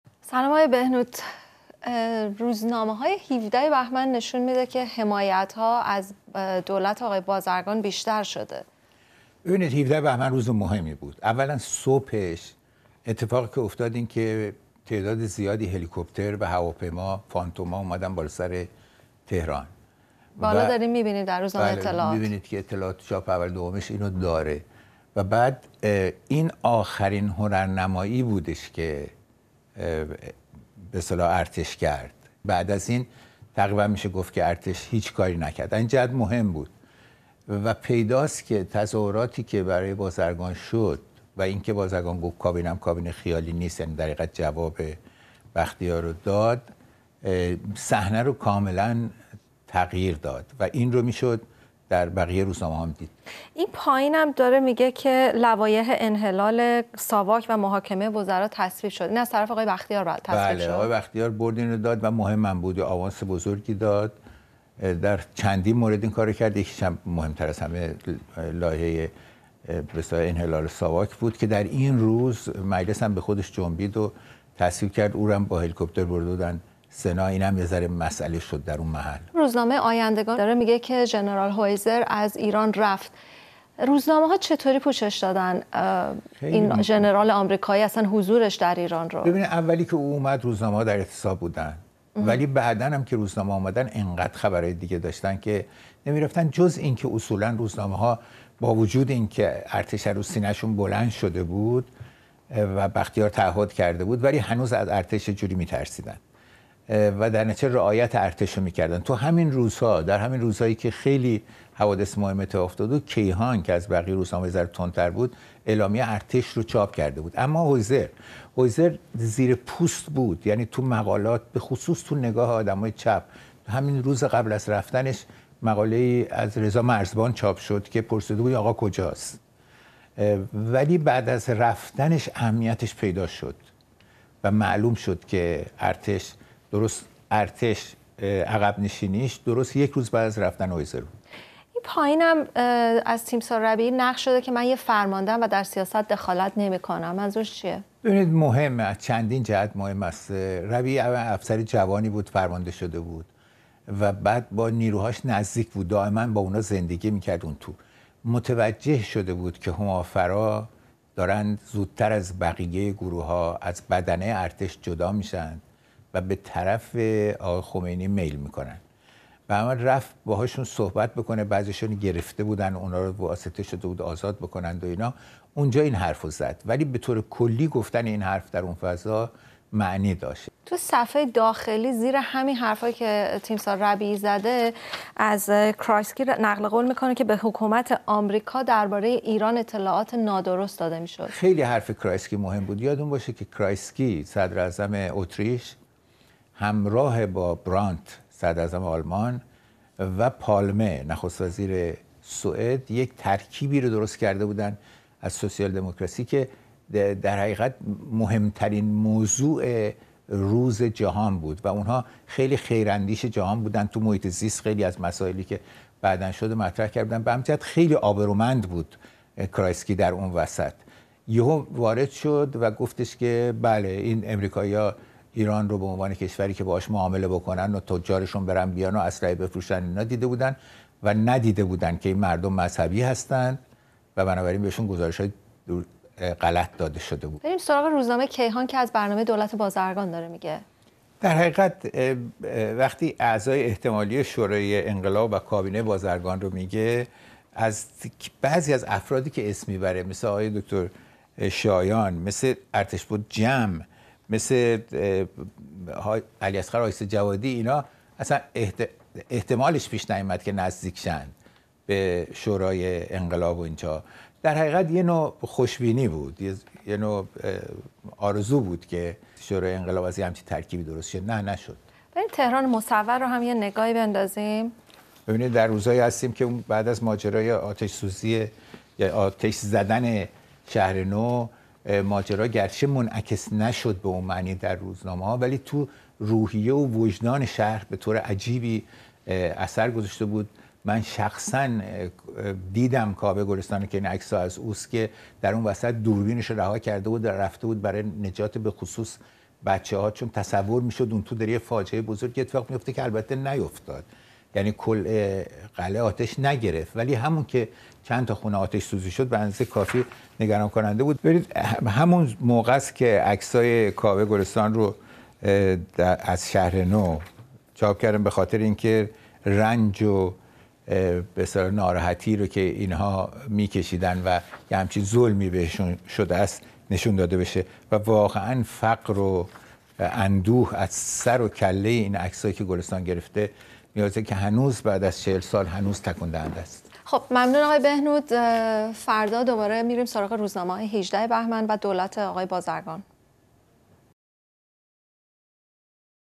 مرور مطبوعات ایران ۱۷ بهمن ۵۷؛ افزایش حمایت از دولت بازرگان
در ادامه مرور مطبوعات روزهای انقلاب ۱۳۵۷، نگاهی می کنیم به مطبوعات ایران در روز ۱۷ بهمن.
مسعود بهنود و مریم افشنگ مطبوعات آن روز را ورق می زنند و نگاهی می اندازند به انعکاس وقایع ۱۷ بهمن ۵۷ در روزنامه های اصلی آن دوران.